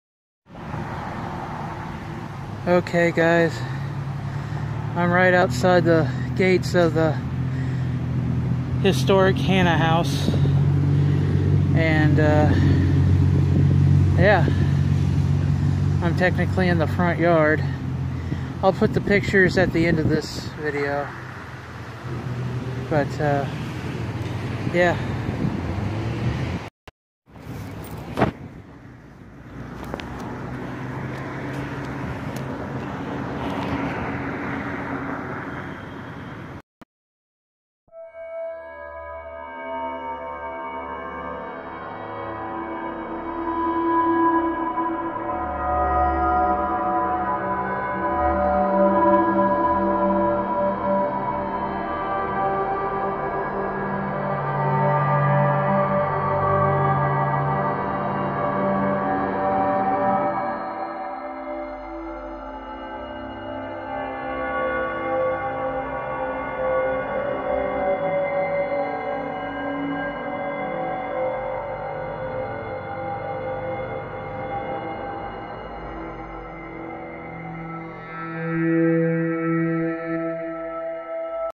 I Went Back To The Haunted House!
Yes,I went back to the haunted Hannah House! What did I see this time? Did anything paranormal happen to me? Did I go missing and this is a found footage video? Let's find out!

If you would like artwork done by Hells Mistress and at a reasonable price.

My  channel is a proud supporter of.